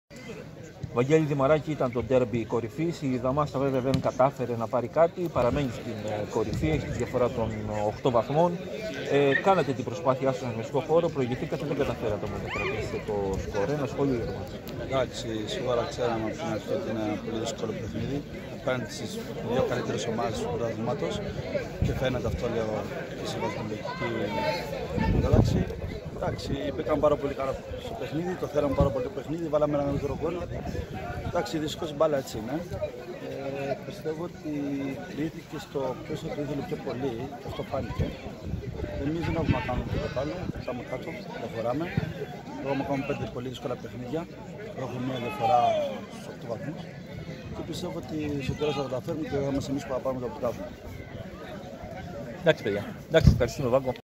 Β. Δημαράκης: «Ξέραμε από την αρχή πως ήταν ένα δύσκολο παιχνίδι»
Α1 ΕΠΣΗ, 21η αγωνιστική.
Αλμυρός-Δαμάστα 2-1